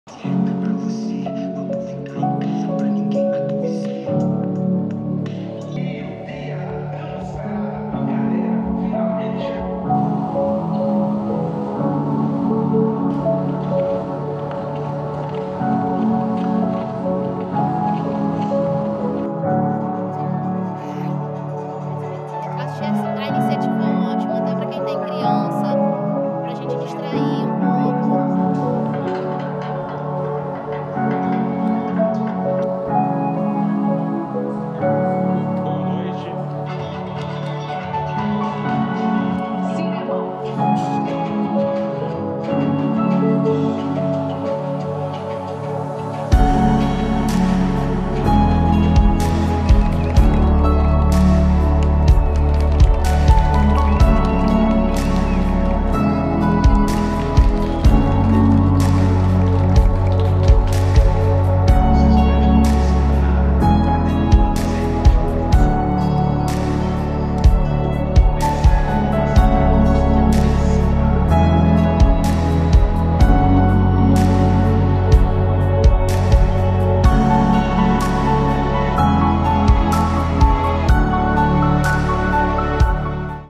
Warga Rio Bisa Nonton Film Lewat Jendela
Layar film portabel besar dipasang di luar gedung apartemen di Rio de Janeiro pada Rabu (13/5). "Cinema in Windows", sebuah inisiatif Balai Kota Rio untuk mendorong orang tetap diam di rumah tapi bersenang-senang selama pandemi.

Orang tua dan anak-anak berkerumum di jendela apartemen untuk menonton film-film pendek yang kebanyakan untuk anak-anak. Ada juga yang menonton di mobil.

Gambar diproyeksikan dari kendaraan utilitas olahraga yang dilengkapi khusus. Audio disiarkan dari SUV, menciptakan pengalaman yang mirip dengan film drive-in.

Sebelum pandemi, kru film seluler RioFilme membawa SUV ke lingkungan di seluruh kota untuk menonton film-film gratis outdoor di taman dan plaza. Tetapi pertunjukan itu ditangguhkan untuk mencegah kerumunan besar orang berkumpul yang dapat menyebarkan virus.

Brasil telah memiliki lebih dari 12.000 kematian akibat COVID-19. Brasil menjadi negara Amerika Latin yang paling terpukul menurut survei oleh Johns Hopkins University.